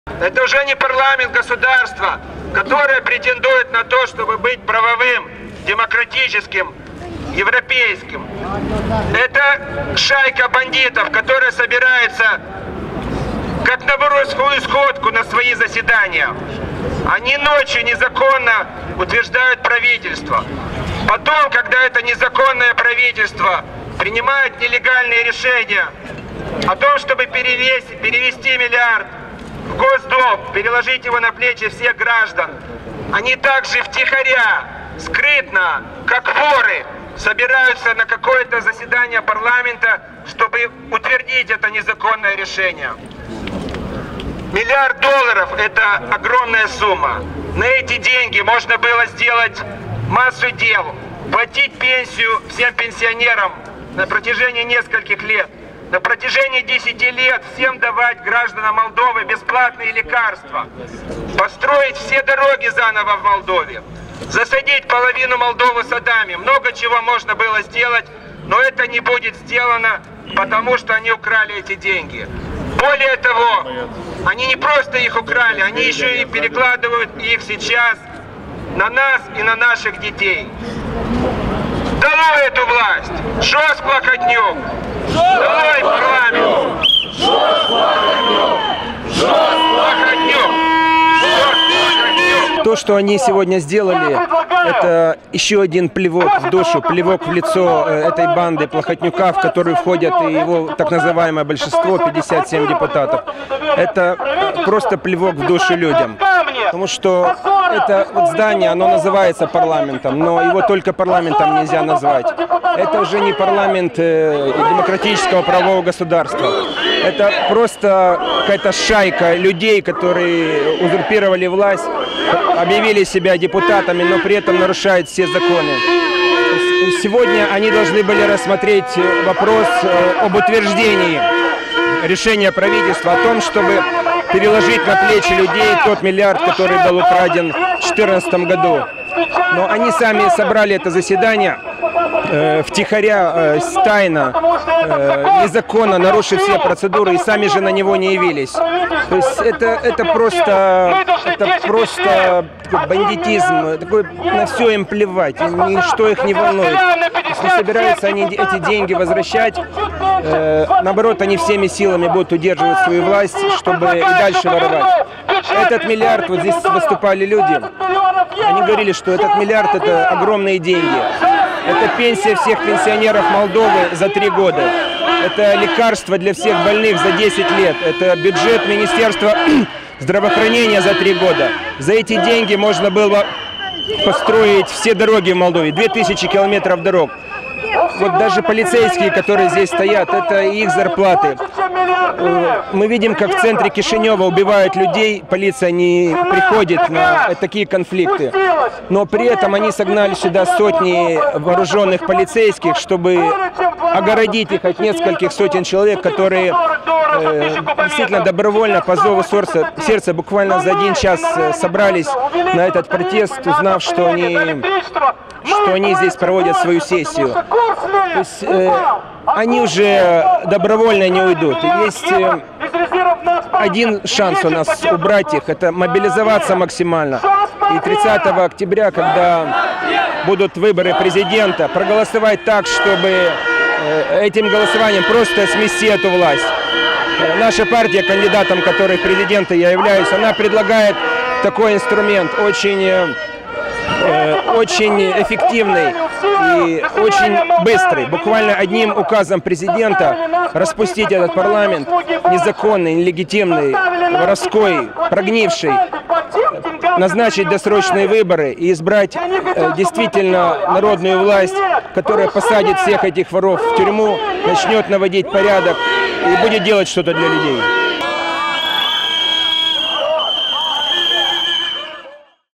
Чубашенко на протесте: Сходка грабителей по кличке “парламент” снова плюнула народу в душу
Кандидат от «Нашей Партии» на должность президента Молдовы Дмитрий Чубашенко принял участие в акции протеста против принятого правительством решения о возложении на плечи граждан страны разворованного властями миллиарда.